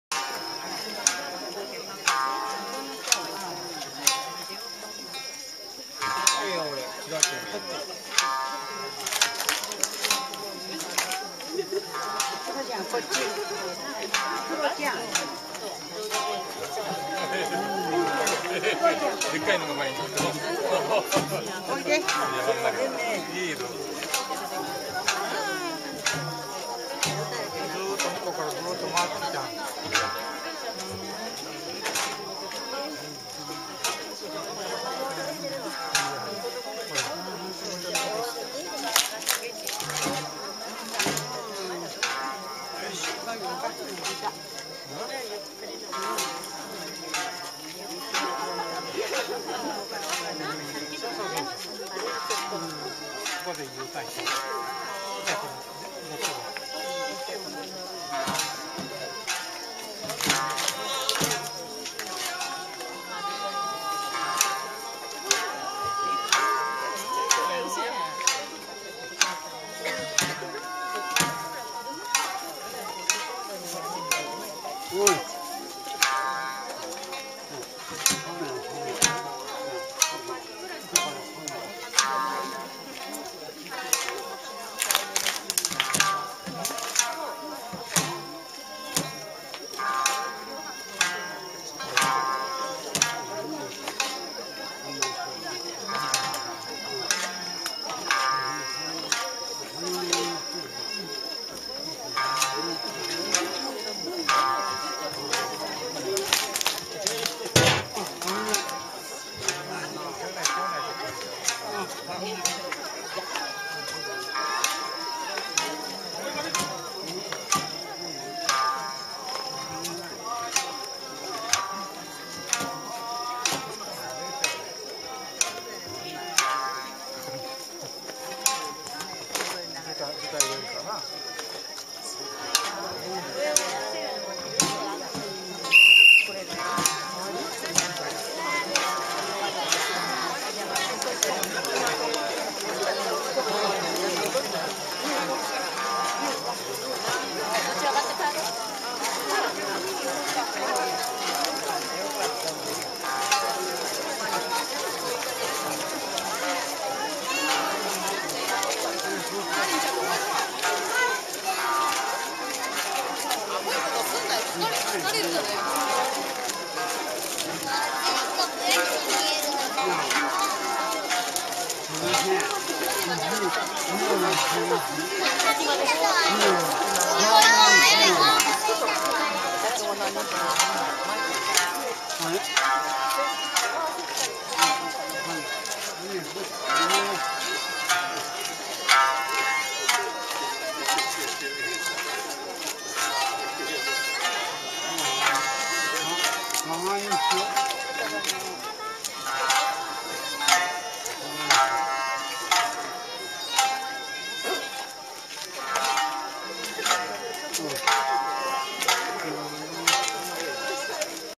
'08 Owara Kaze no Bon14 / おわら風の盆14〜鏡町輪踊り
I went to see "Owara Kaze no Bon(Bon festival of Owara or Bon Dance of the Wind)" held in Etchu Yao, Toyama Prefecture from September 1, 2008 to the 3rd. I took a movie of the dance of the dancer in Kagami-machi on September 2.

2008年9月2日、越中八尾のおわら風の盆にて。踊っているのは鏡町の方々です。
あまりに暗いところだったので、明るさを無理矢理上げました。